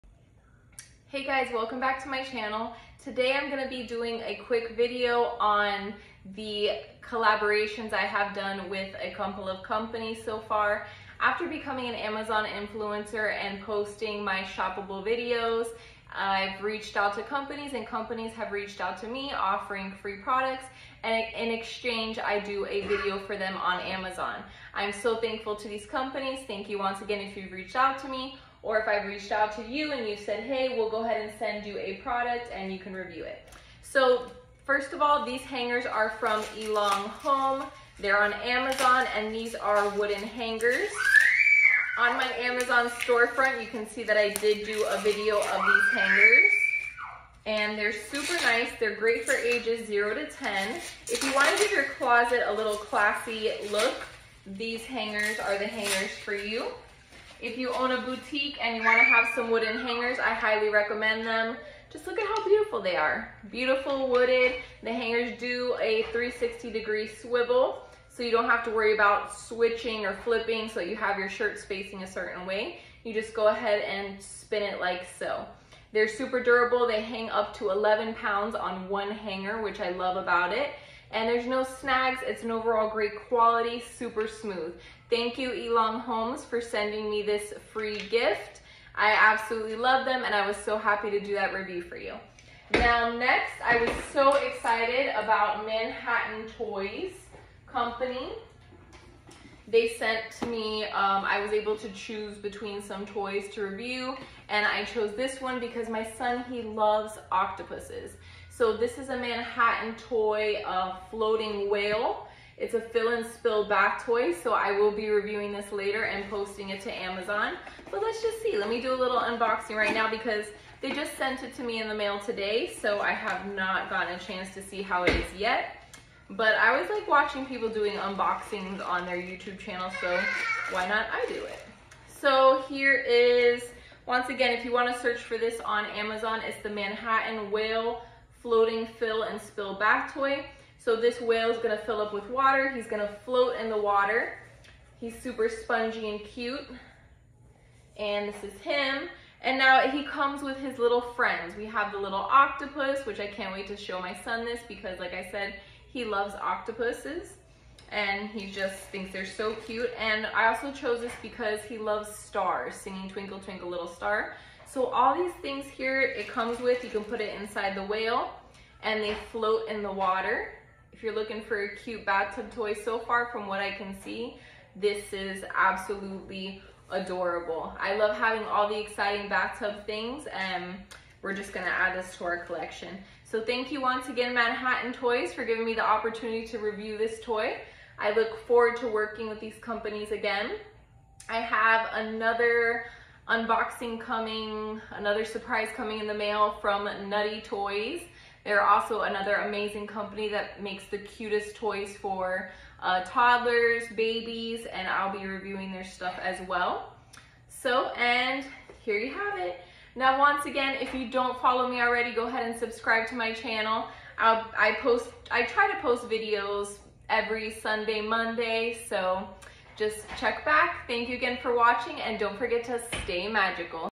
Elong home wooden baby hangers & Manhattan toy bath whale
Reviewing the Elong home wooden baby hangers & Manhattan toy neoprene whale spill & fill

Link to these products are below ⬇️